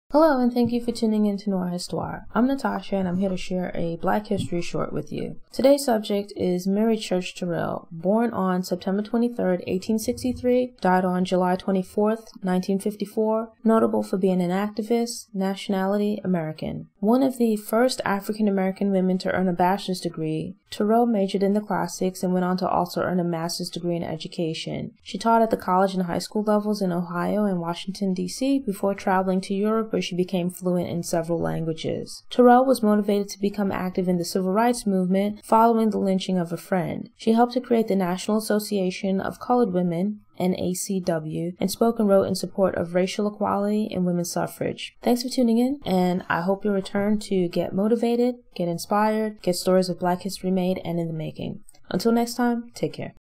Mary Church Terrell: Racial equality and women's suffrage activist | Black History Facts
A profile of Mary Church Terrell who was one of the first African-American women to earn a Bachelor’s degree, helped to create the National Association of Colored Women (NACW), and spoke and wrote in support of racial equality and women's suffrage.